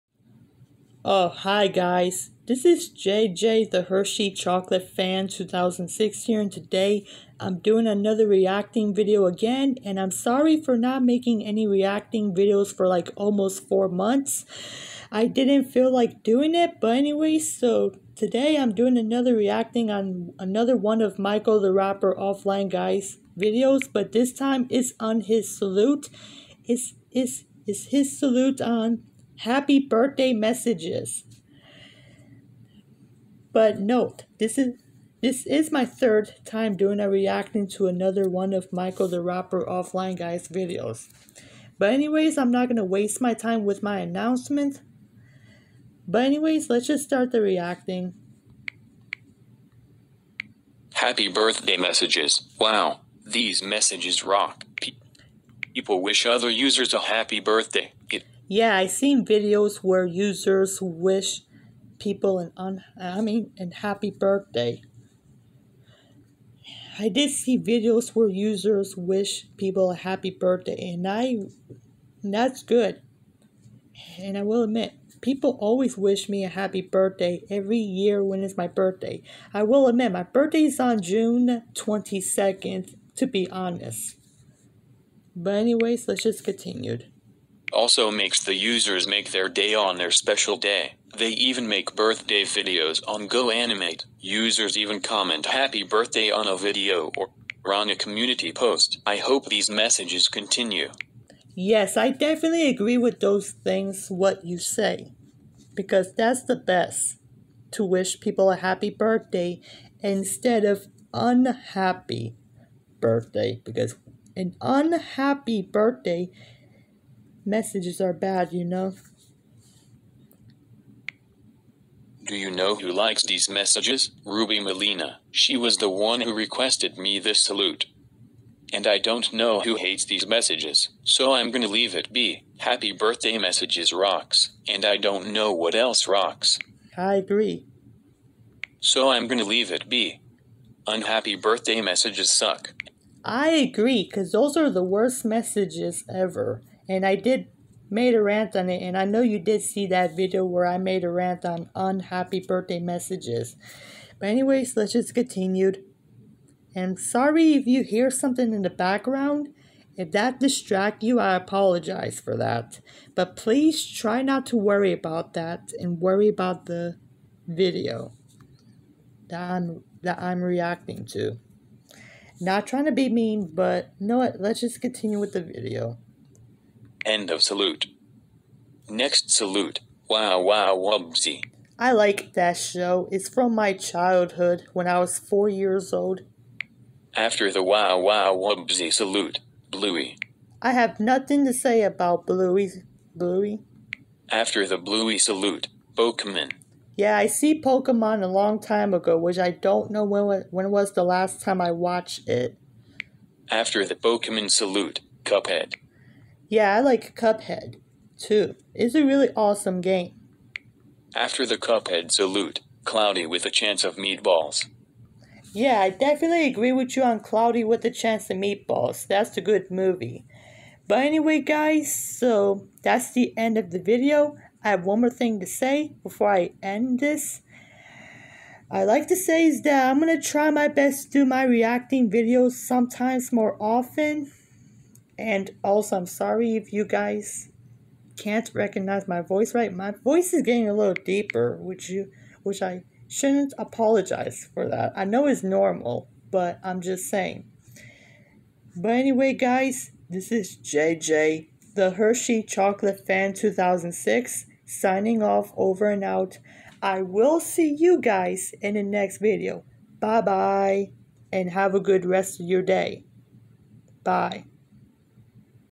JJ Reacts #5: Michael The Wrapper Offline Guy Salute on Happy Birthday Messages
note: I'm sorry for not doing any reacting videos for almost 4 months I didn't feel like doing it but I'm going to try my best to do it sometime more often.